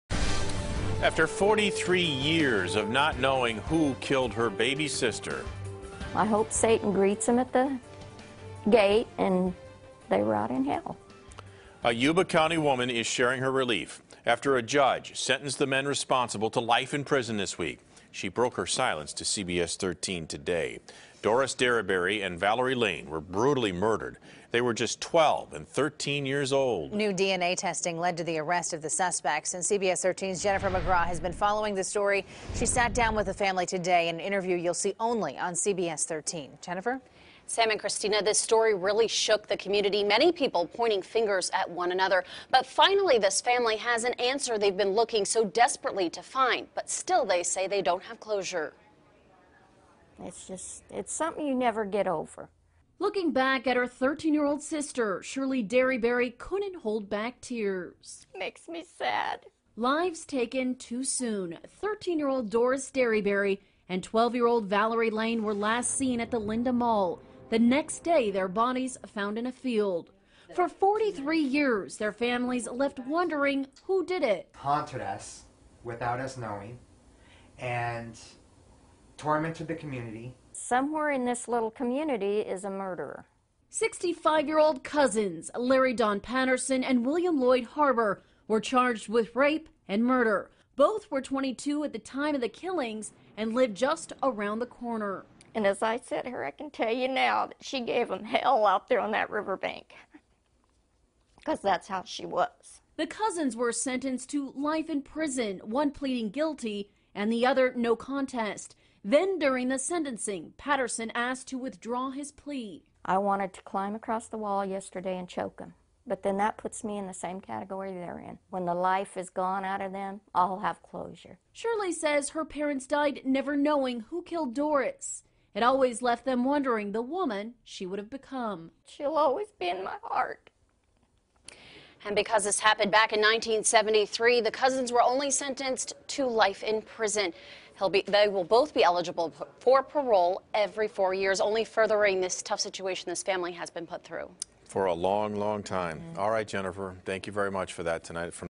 Woman Relieved After Sister's Killers Sentenced, 43 Years Later
Lives taken too soon, 13-year-old Doris Derryberry and 12-year-old Valerie Lane were last seen in 1973 at the Linda mall. The next day their bodies found in a field.